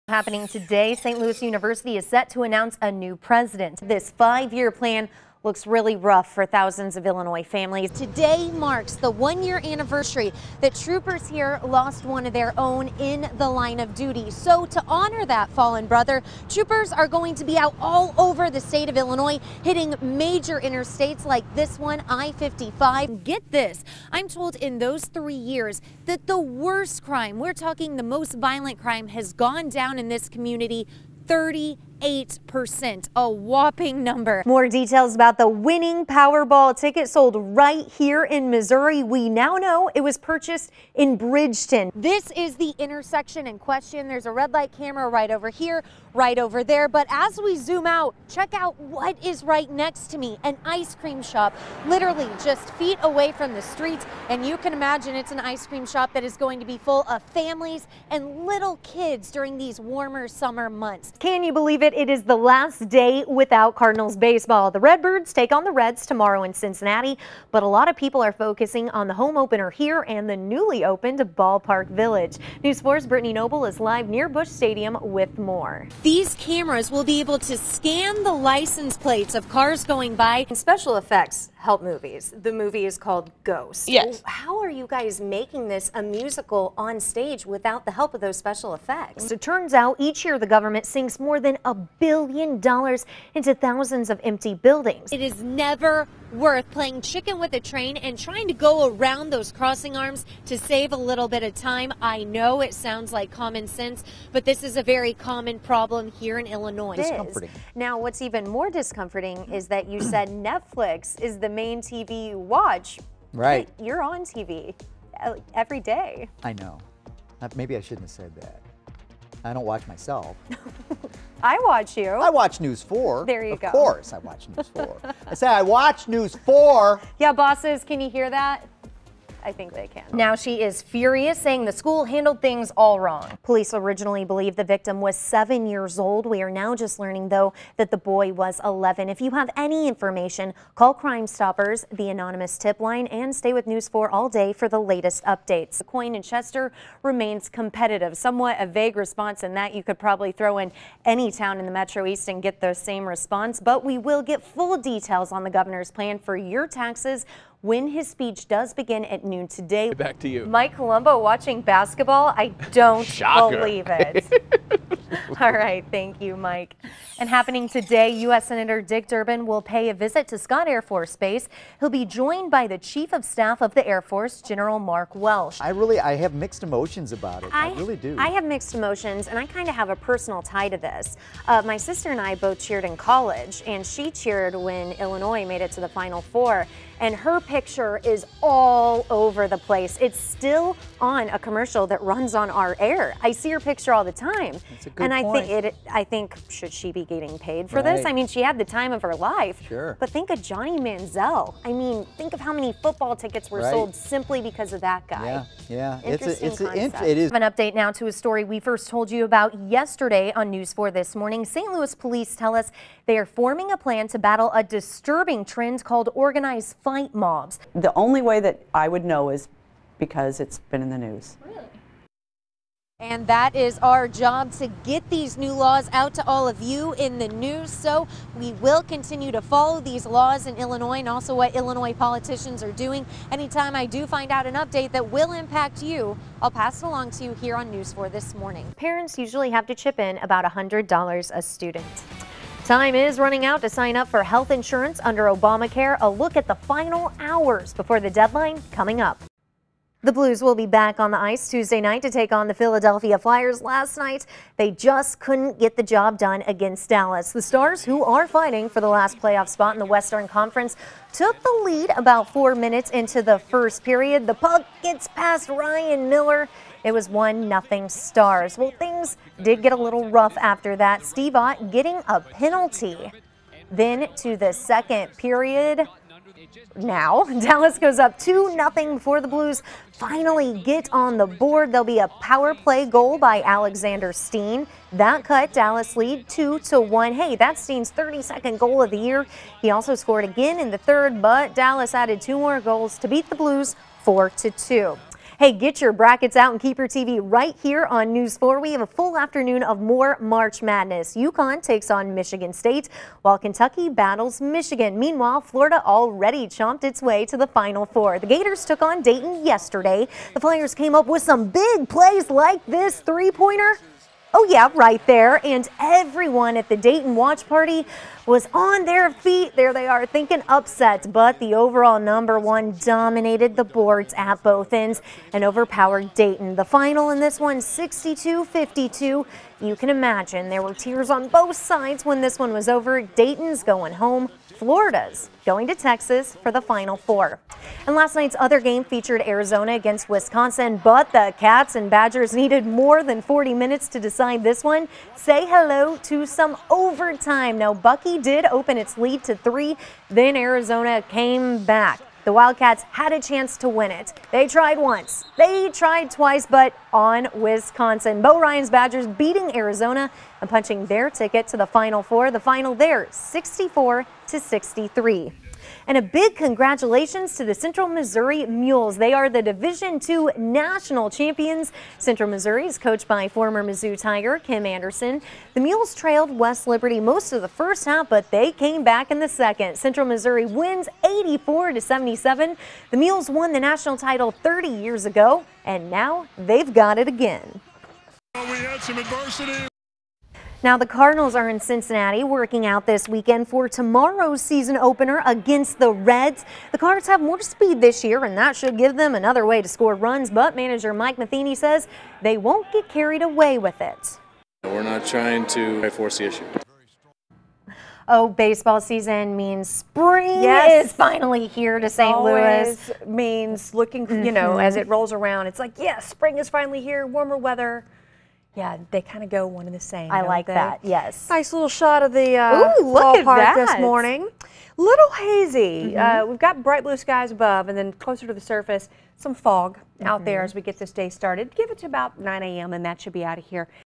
3/20-3/30 Live Work
Laura Hettiger's broadcast work from March 20-30: anchoring, hosting and live reporting